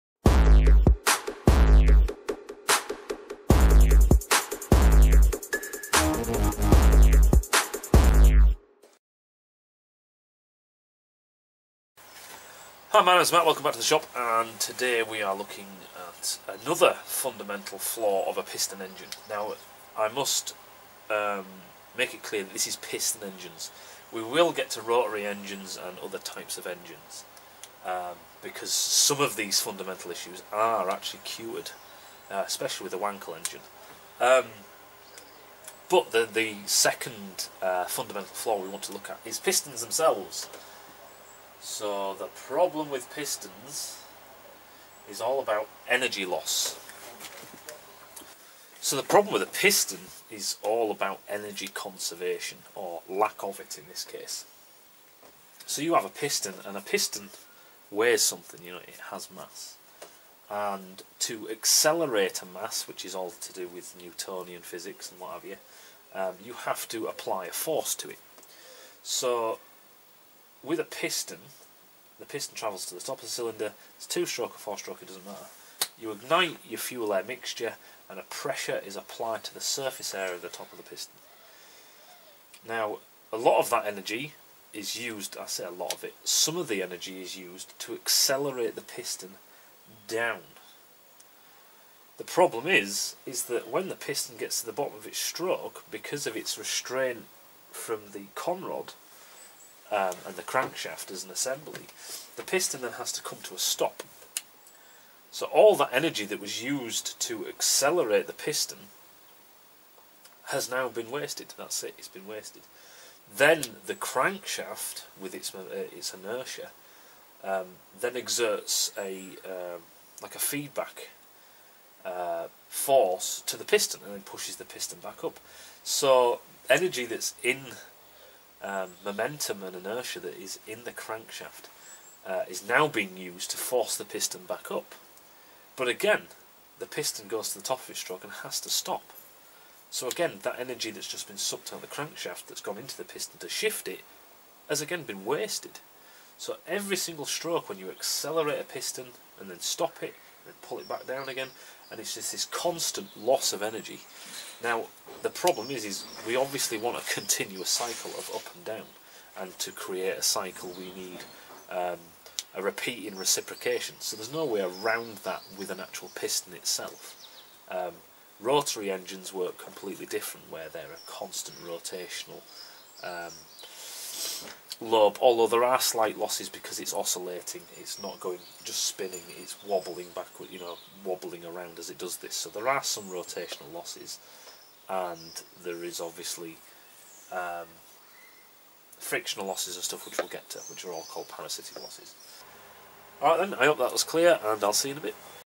Engine Fundamental Flaws - Part 2 Pistons
In this episode we look at the piston and what inherent flaw this has.